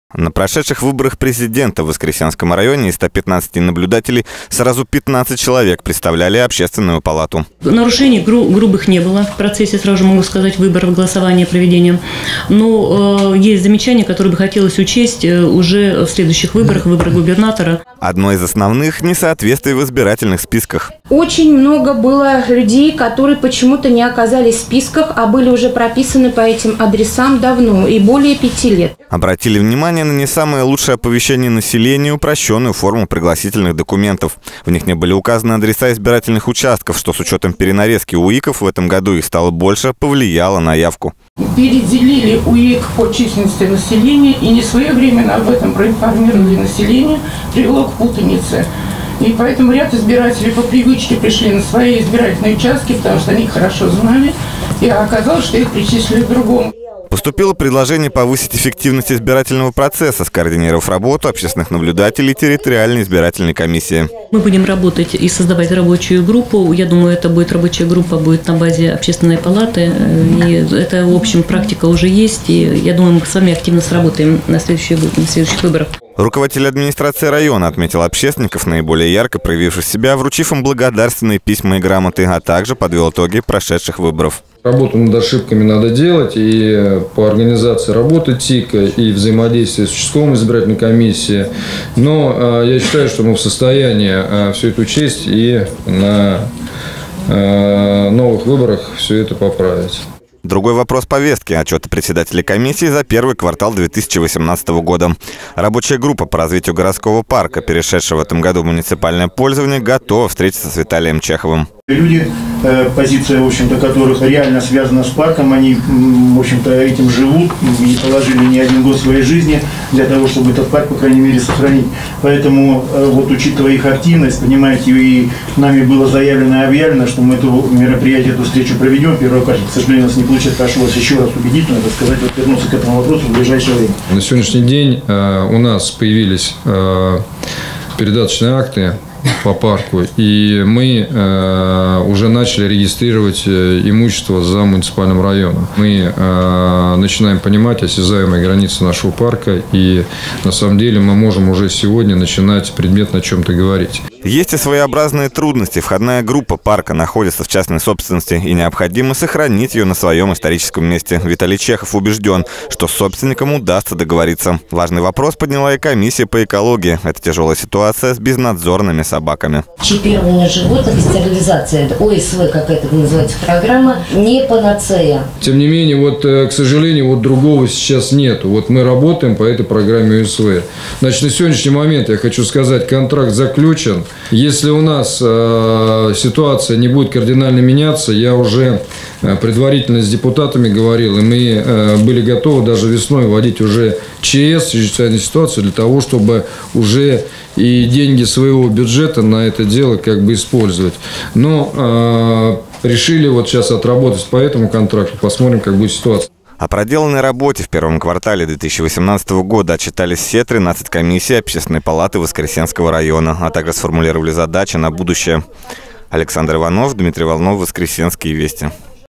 Итоги выборов, благоустройство парка и безнадзорные собаки. Пленарное заседание Общественной палаты
На очередном пленарном заседании Общественной палаты Воскресенского района обсудили работу наблюдателей на прошедших выборах президента. Главы профильных комиссий отчитались за первый квартал этого года.